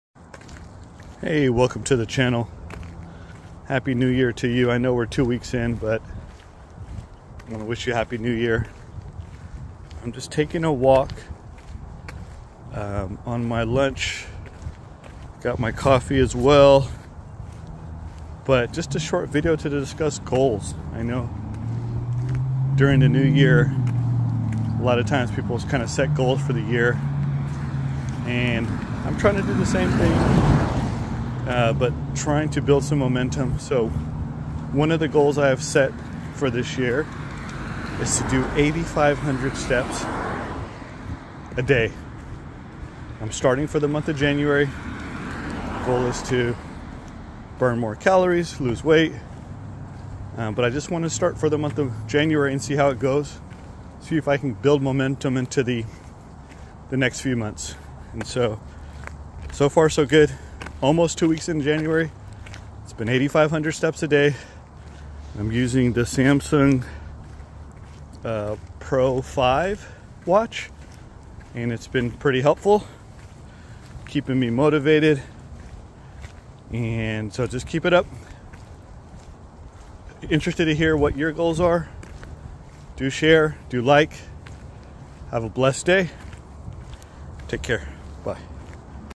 Keep walking! Daily step goals on Samsung Pro 5
8500 steps per day in the month of January